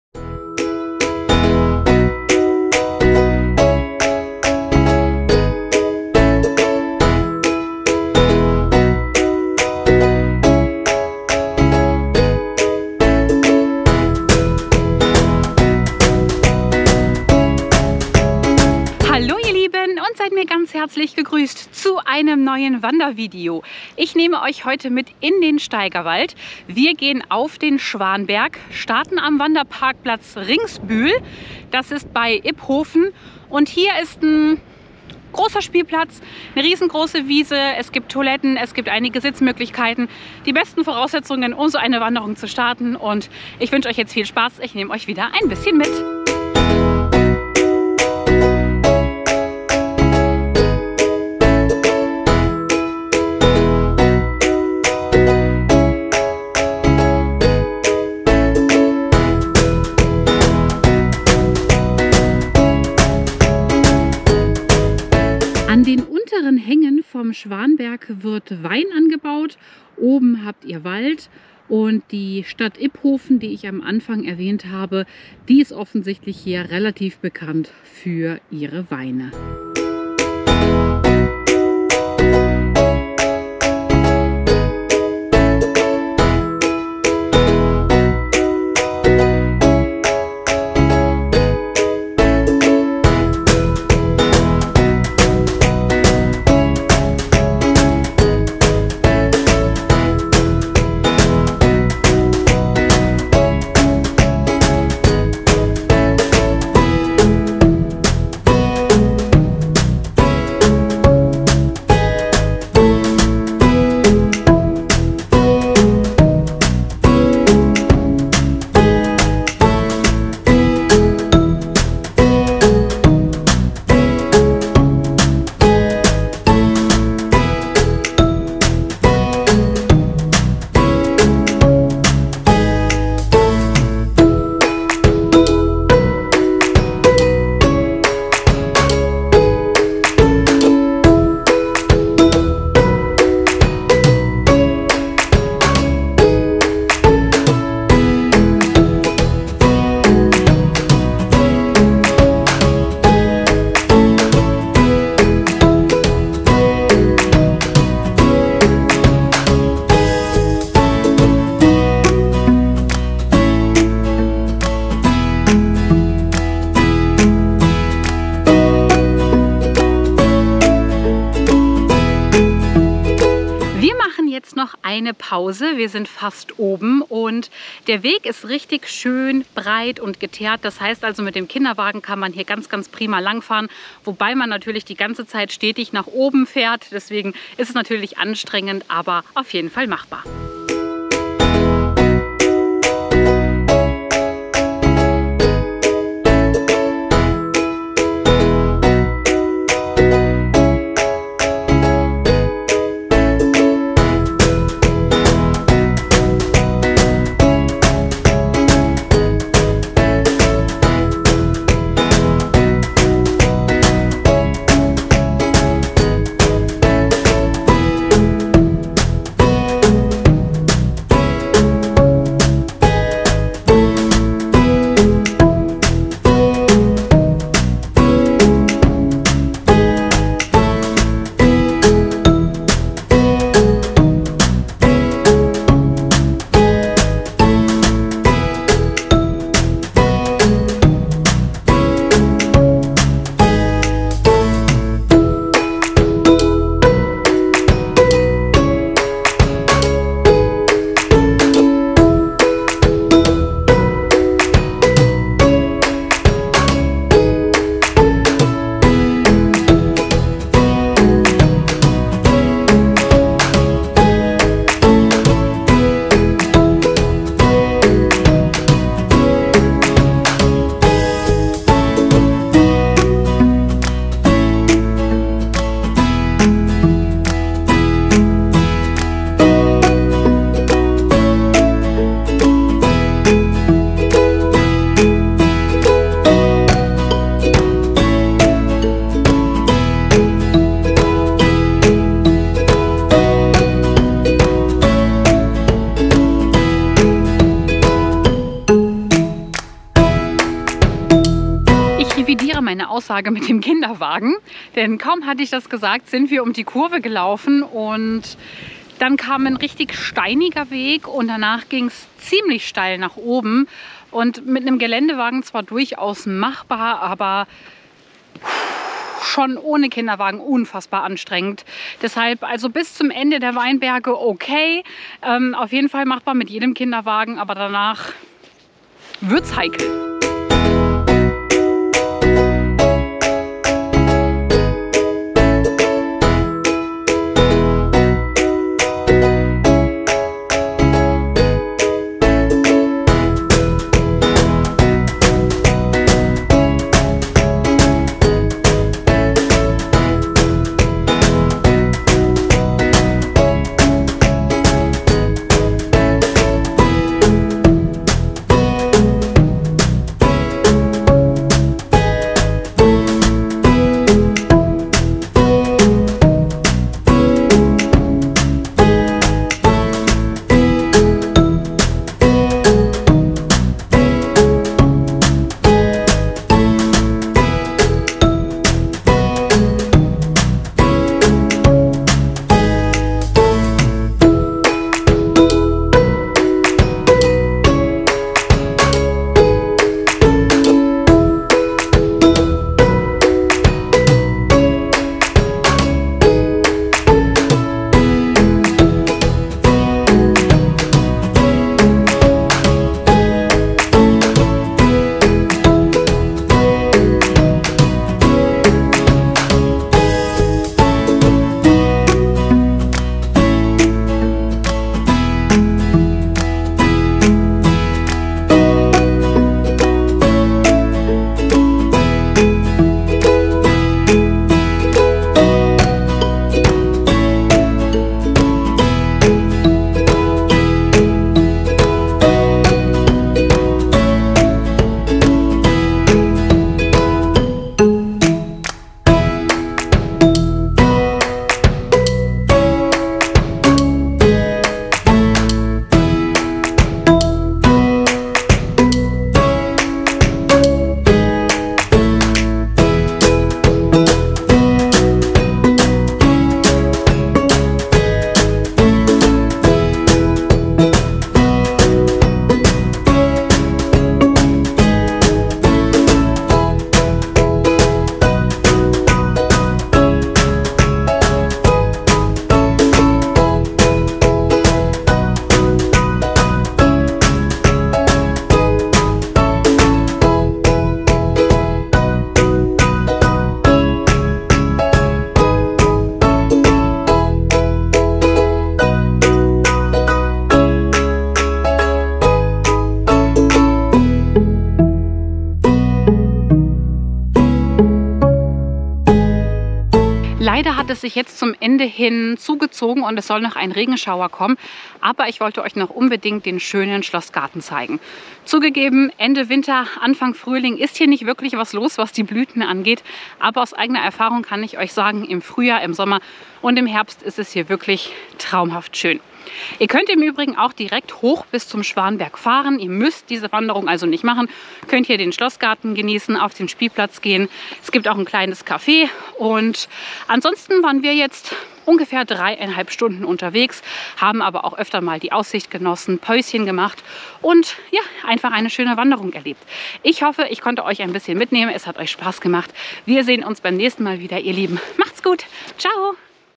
Wanderung auf den Schwanberg (Steigerwald)
Ich nehme euch mit zu einer Wanderung auf den Schwanberg in den Steigerwald. Wir starten bei herrlichen Wetter und besten Vorraussetzungen für die Natür.

Zeitstempel:
00:00 - Intro
00:18 - Begrüßung
00:45 - Wanderung auf den Schwanberg
04:47 - Auf dem Schwanberg
06:44 - Schlosspark Schwanberg
08:20 - Verabschiedung

Kamera: GoPro Hero 9 black
Schnittprogramm: Adobe Premiere Elements 2021